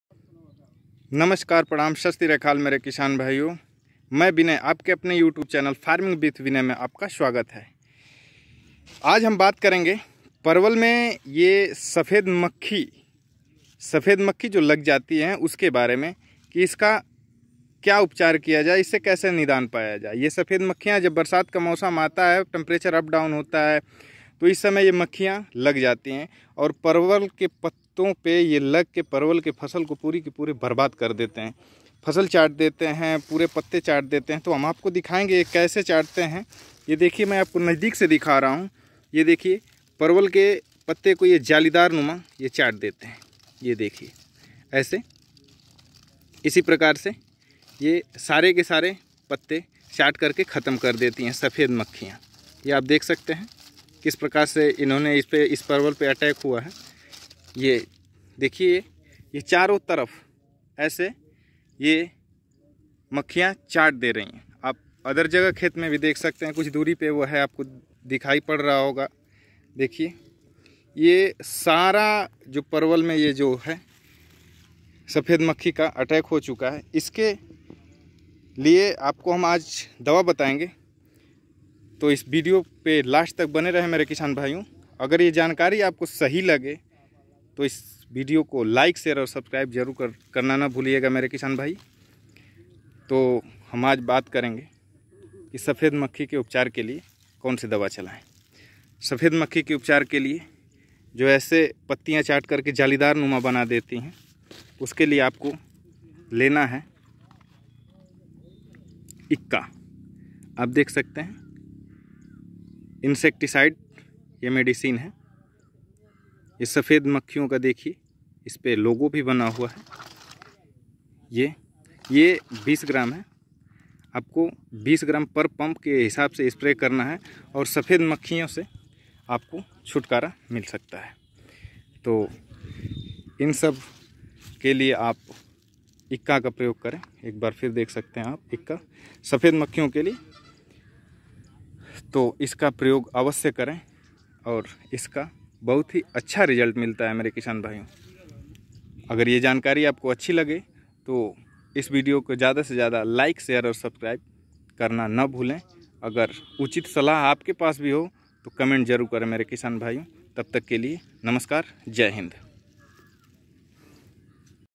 परवल में सफेद मक्खी से पाए तुरंत छुटकारा parval me saphed makhi se paye turant chutkara
नमस्कार मेरे प्रिय किसान भाइयों
इस वीडियो में हमने सफेद मक्खी के बारे
में बताया है
farming with vinay
अगर ये वीडियो आप लोगो को अच्छी
मेरे किसान भाइयों
नमस्कार जय हिंद